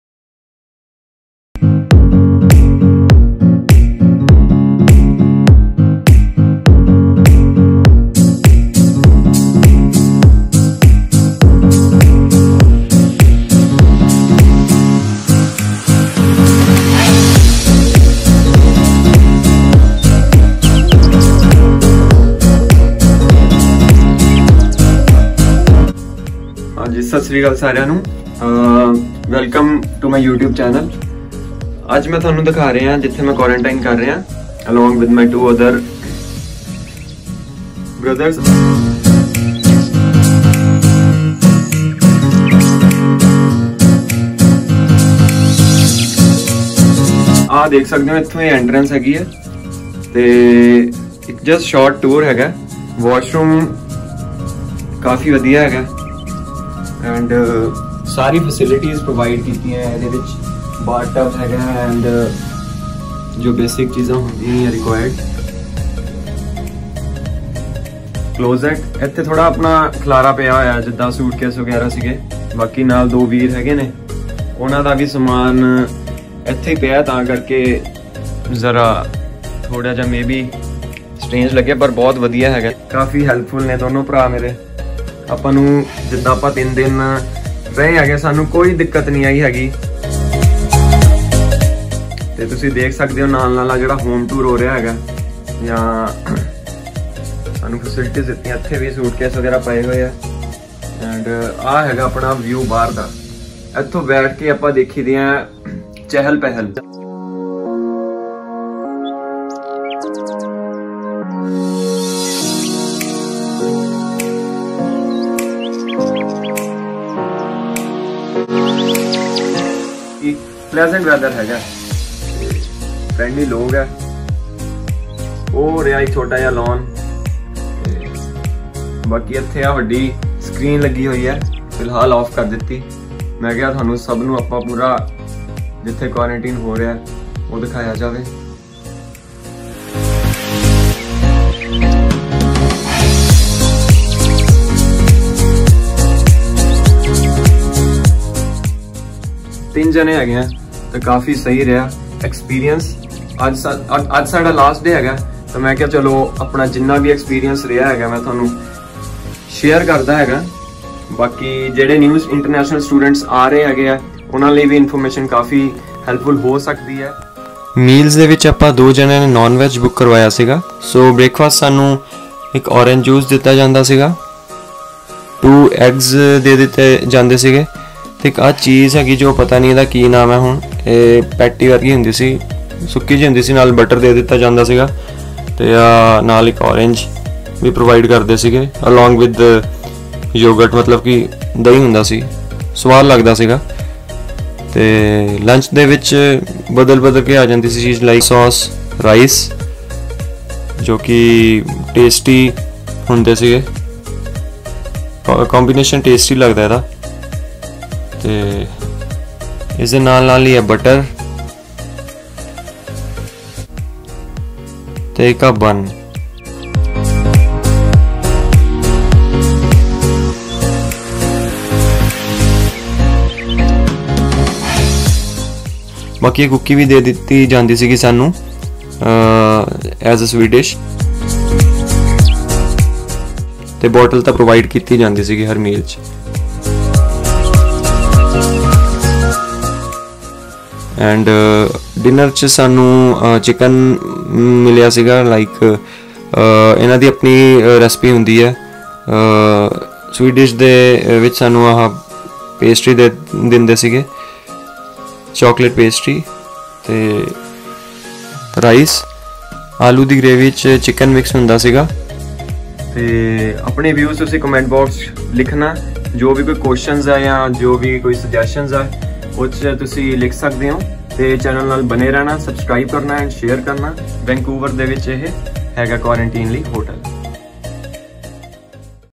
3-Days CANADA 🇨🇦 quarantine experience in cheapest hotel
In this video, showing my 3-days quarantine journey in GAA Canadian hotel . Total cost was $1476 (CAD) for three people.